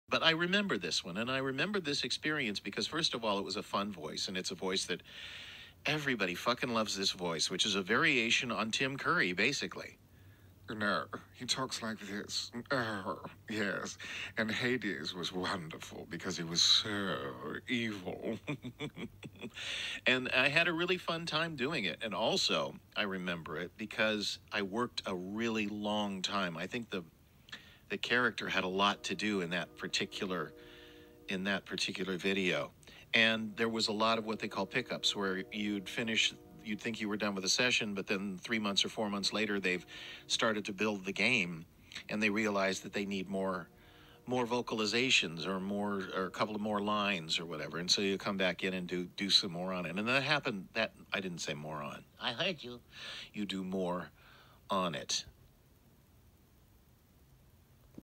Voice of Hades (Kid Icarus: Uprising) Scott Bullock
Hear the voice actor for Hades (S Scott Bullock) talk about his experience voicing the lord of the underworld. Enjoy :D

Kid Icarus uprising by Nintendo